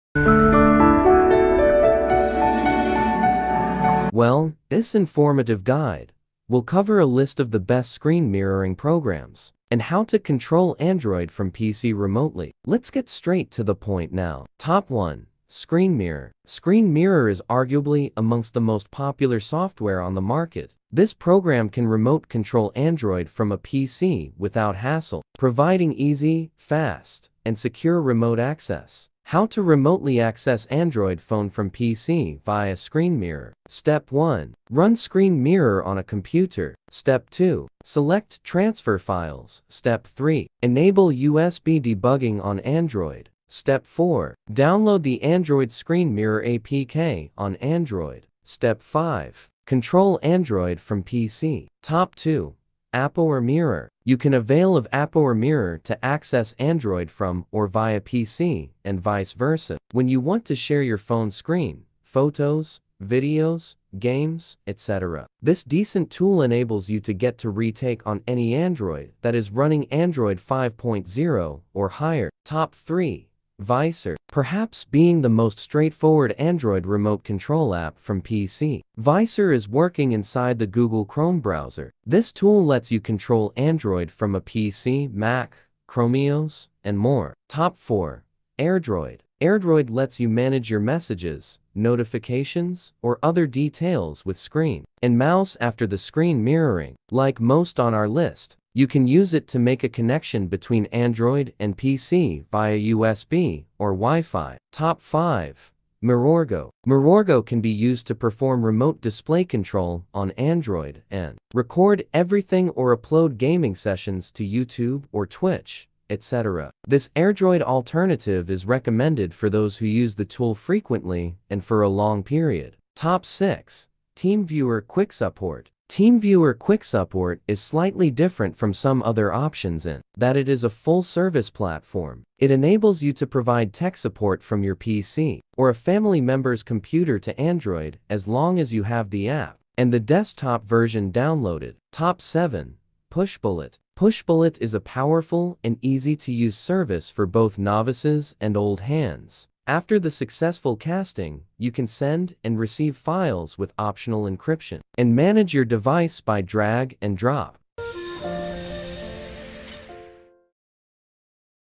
Control Android from PC with 7 Best Screen Mirroring Apps
Any ideas on how to use your Android phone on your computer remotely? Check out this video with the top 7 best screen mirroring software/apps. Then you can pick anyone to Control Android from PC.